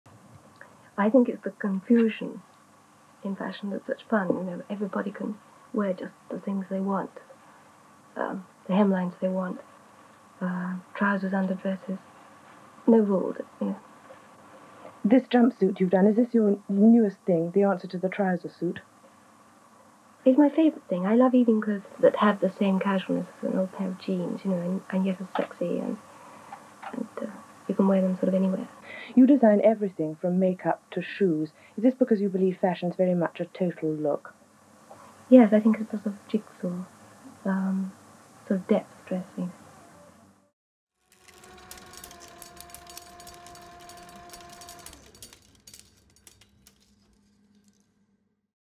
"No rules" – Mary Quant on the Ideas Behind her Iconic Designs (1969)
“Everybody can wear just the things they want... No rules.”

Mary Quant, the iconic British designer and driving force behind the groundbreaking fashion of the Swinging Sixties, has died aged 93.

In 1969, Quant spoke to ITN about the ideas that guided her innovative designs.